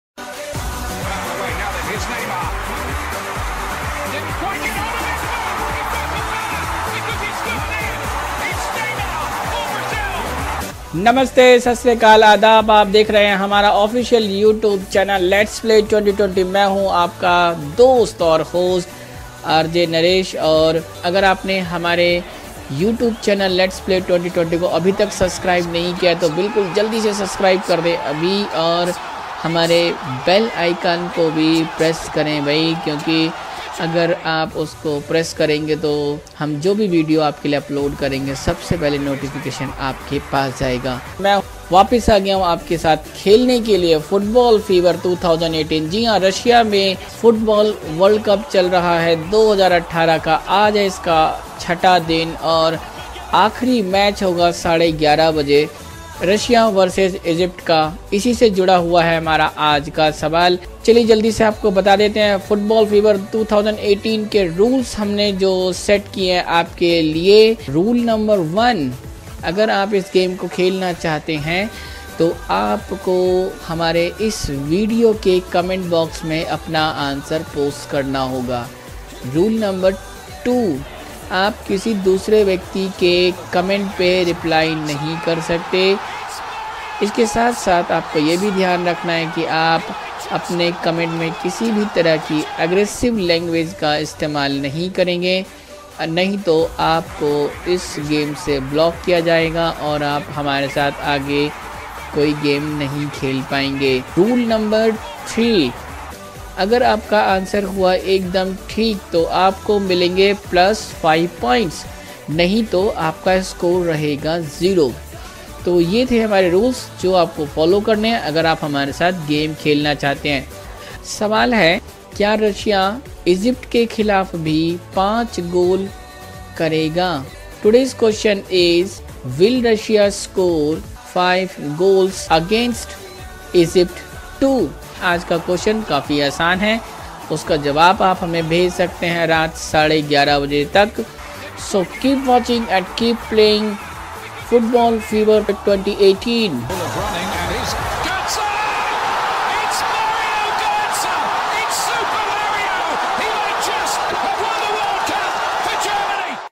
RUSSIA VS EGYPT GROUP A QUESTION TO PLAY FOOTBALL FEVER 2018 WITH RJ NARESH
"play football fever 2018"
"WIN EXCITED SURPRISE GIFTS"
"RUSSIA VS EGYPT"
Football world  up 2018 day6 june19 2018 group A match between russia and egypt at 11.30pm today. Question for this match is:
"WILL RUSSIA SCORE 5 GOALS AGAINST EGYPT TOO?"
Post your answer in comment box till 11.30pm today. keep playing with us
"FOOTBALL FEVER 2018"